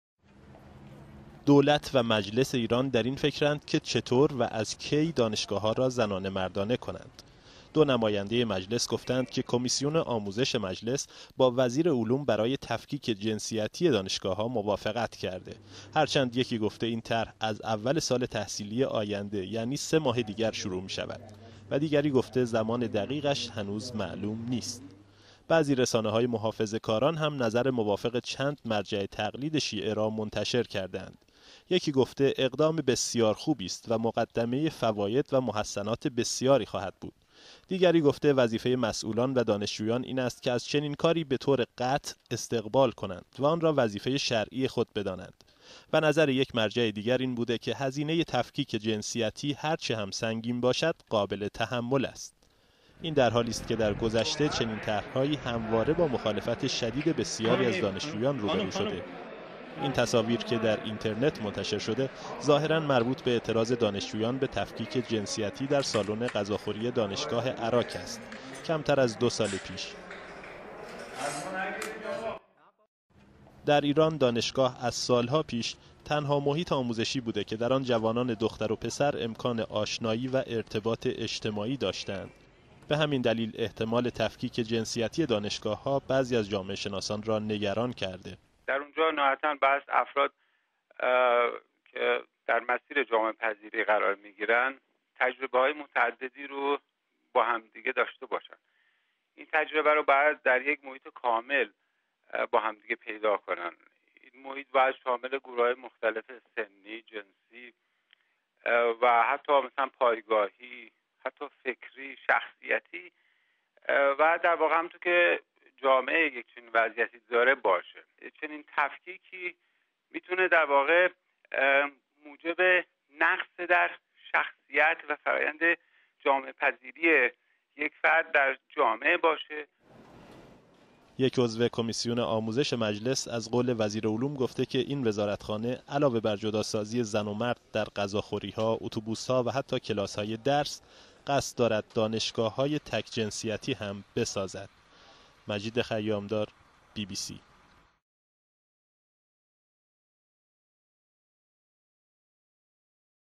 Iran - 22 June 2011 sex segregation Debate in universities has intensified
شدت گرفتن بحث تفکیک جنسیتی دانشگاه‌ها در ایران
بحث تفکیک جنسیتی دانشگاه‌ها در ایران قوت گرفته. هر چند زمان دقیق جدا کردن دانشجویان دختر و پسر در دانشگاه‌ها هنوز به روشنی معلوم نیست، اما بعضی نمایندگان مجلس از توافق با وزارت علوم برای اجرای این طرح خبر داده‌اند.